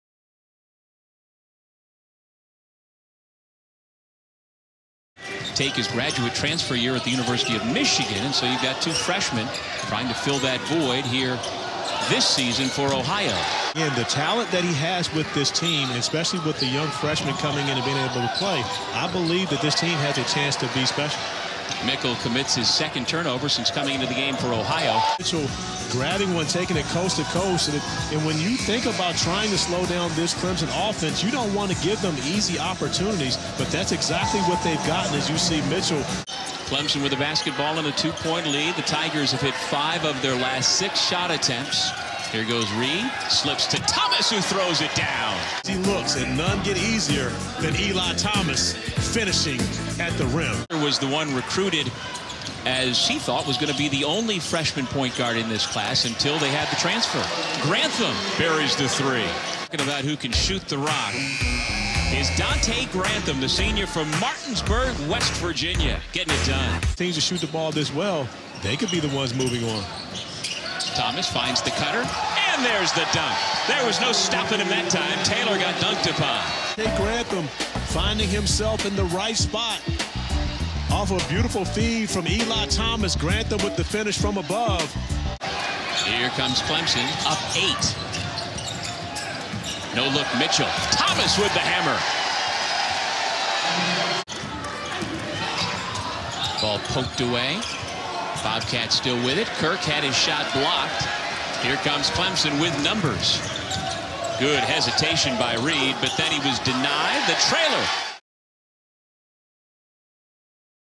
Clemson vs. Ohio Basketball Highlights (2017)
Clemson vs. Ohio: Forward Elijah Thomas scored a career-high 17 points to go along with 15 rebounds as the Tigers upended the Bobcats 81-76 in the Gildan Charleston Classic. Donte Grantham scored a team-high 20 points for Clemson in the win.